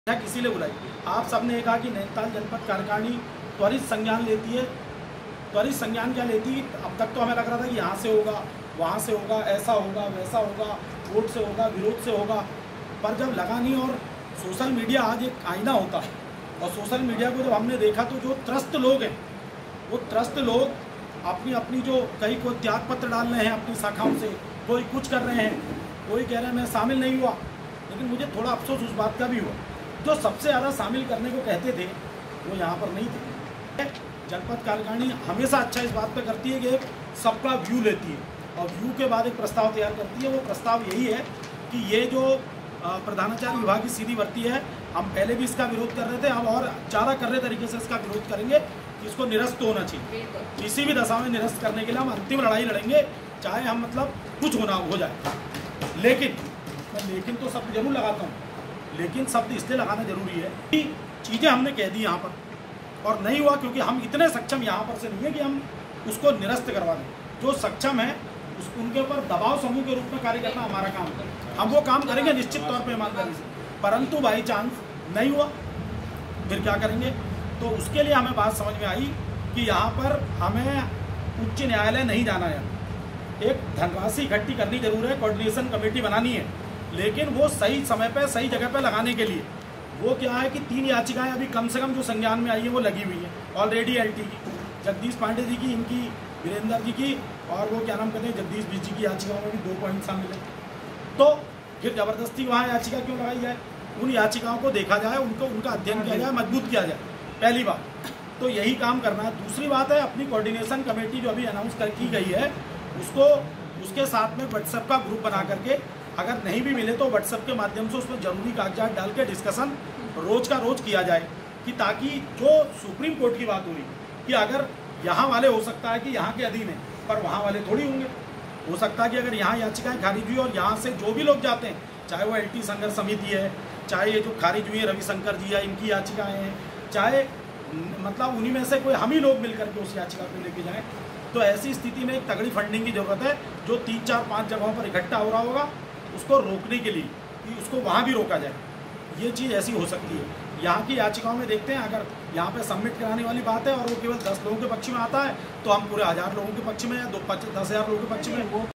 प्रधानाचार्य सीधी भर्ती विरोध : रा०शि०सं० शाखा नैनीताल कार्यकारिणी का उग्र विरोध
प्रधानाचार्य भर्ती को लेकर राजकीय शिक्षक संघ शाखा नैनीताल के द्वारा सबसे पहले विरोध प्रदर्शित किया गया इसके संबंध में एक सामान्य बैठक आयुष की गई जिसमें तय किया गया की शिक्षा विभाग के आईटी संपर्क के शिक्षकों के समर्थन में प्रधानाचार्य सीधी भर्ती का सीधा विरोध किया जाएगा एवं शिक्षा विभाग के सबसे बड़े संपर्क को इस भर्ती परीक्षा में शामिल करने की पूर मांग की जाएगी।